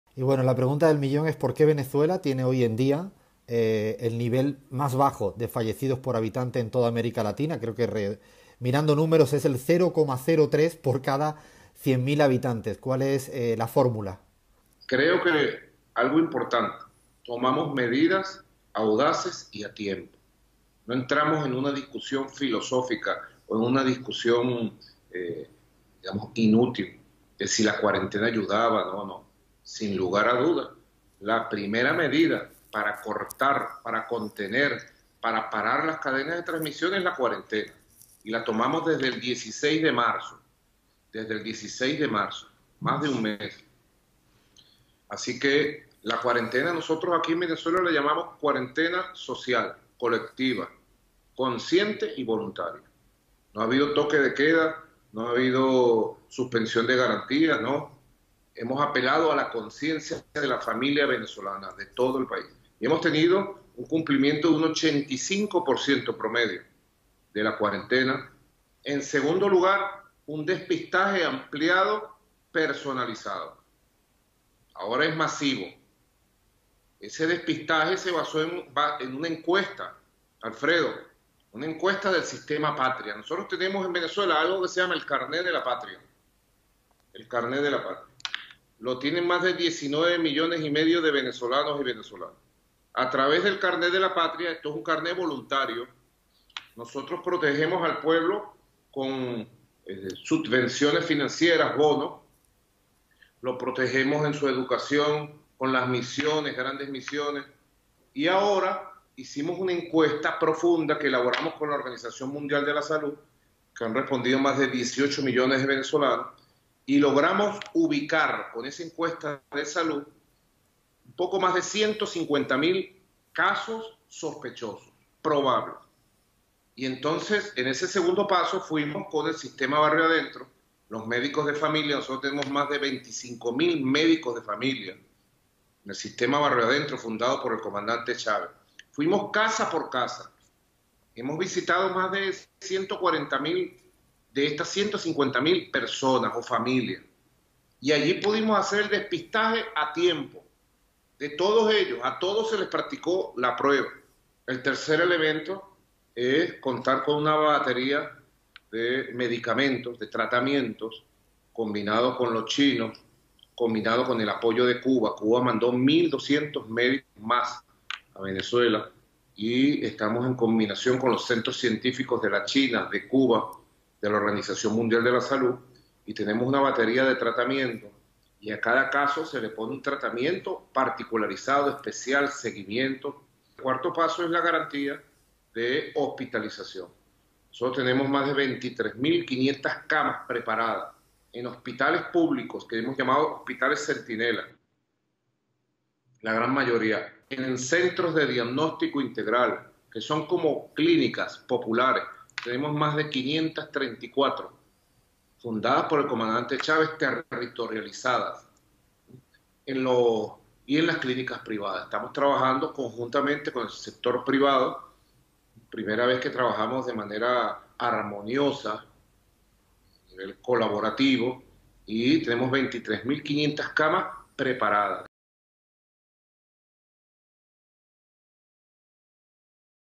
Venezuela Explains Why Its Covid-19 Rates Have Been So Low
Snapshot | Venezuela Explains Why Its Covid-19 Rates Have Been So Low